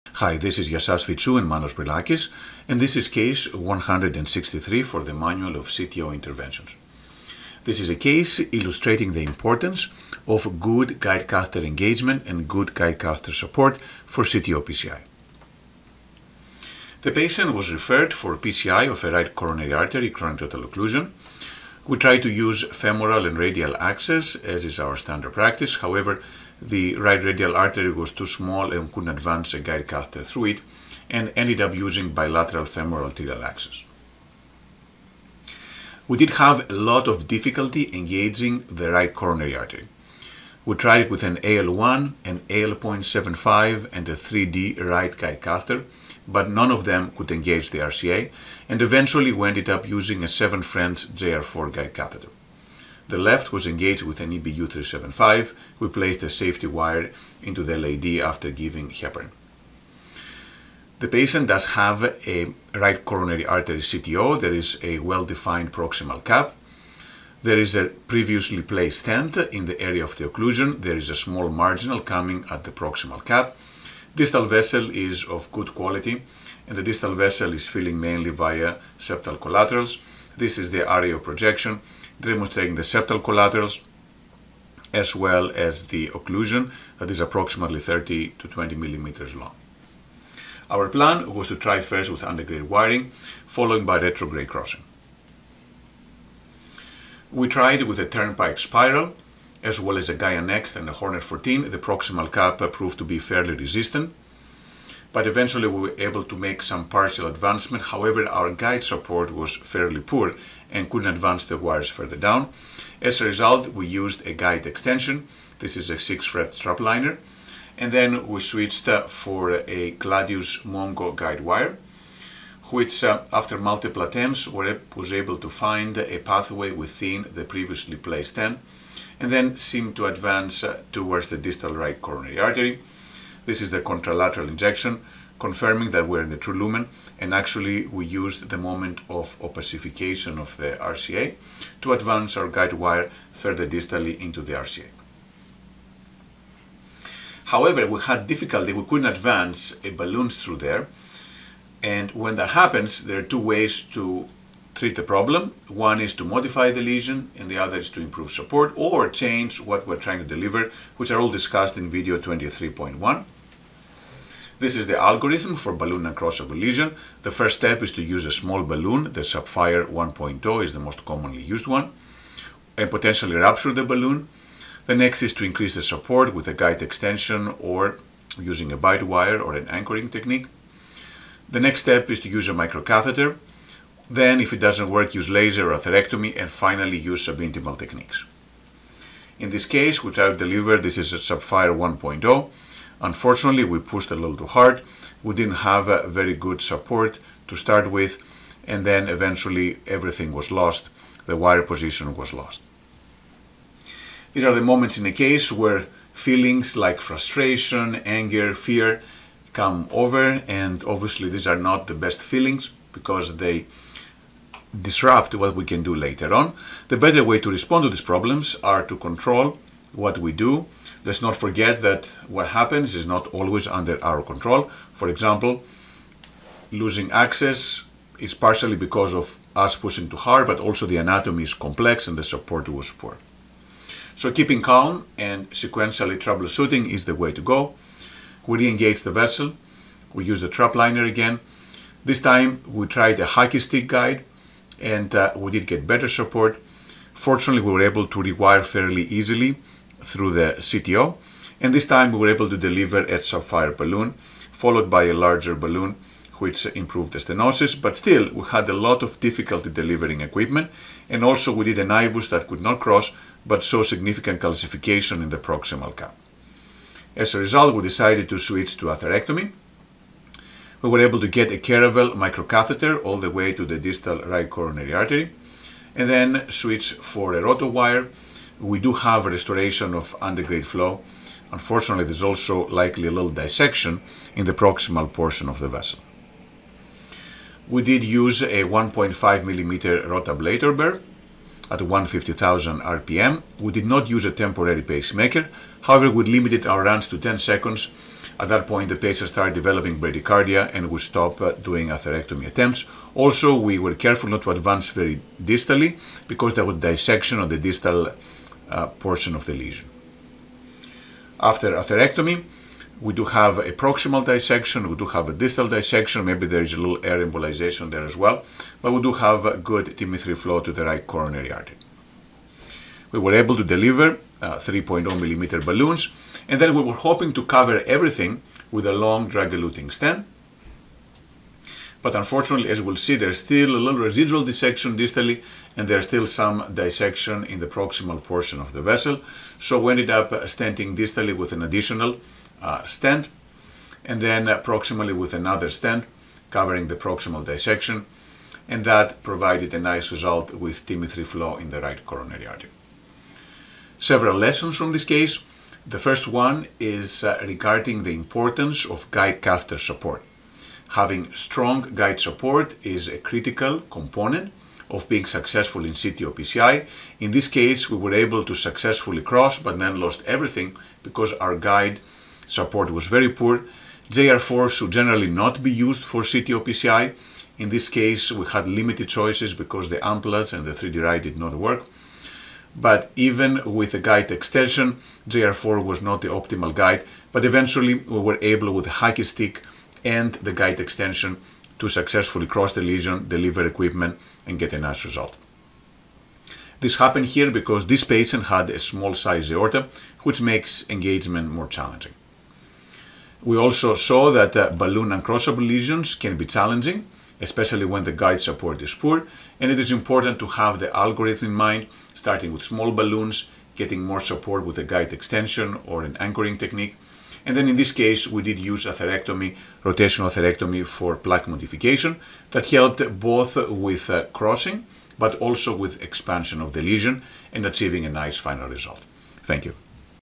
Case 163: Manual of CTO PCI - Engagement issues
A patient was referred for PCI of a RCA CTO due to in-stent restenosis. Engagement of the RCA was very challenging likely due to small size of the ascending aorta. We were unable to engage with an AL1, AL 0.75 and 3D Right guide catheter. We engaged the RCA with a 7 French JR4 guide catheter. Antegrade wire escalation failed with Gaia Next 3 and Hornet 14 in part due to poor guide catheter support. After inserting a 6 French Trapliner we were able to successfully cross the CTO with a Gladius Mongo guidewire, but had difficulty delivering even a 1.0 mm balloon through the lesion and lost guide and wire position. We eventually re-engaged with a Hockey Stick guide and a Trapliner and were able to rewire the lesion and cross it with a 1.0 mm Sapphire Pro balloon. There remained difficulty advancing equipment and IVUS showed severe calcification at the proximal cap. We performed rotational atherectomy with a 1.5 mm burr, followed by successful stenting.